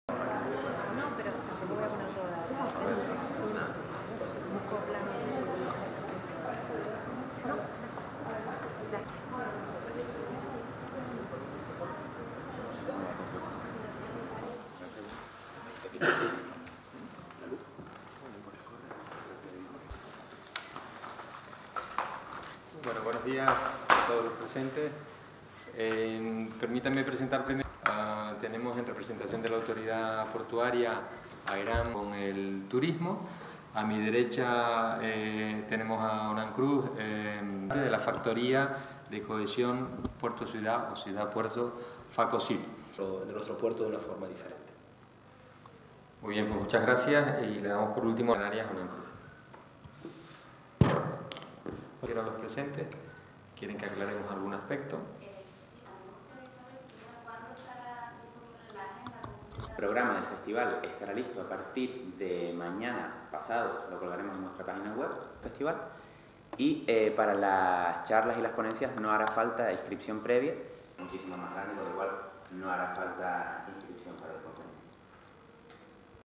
Presentación en Tenerife del Festial Santacruceros
El Cabildo de Tenerife apoya el Festival Santacruceros destinado a potenciar el nexo puerto-ciudad en la capital tinerfeña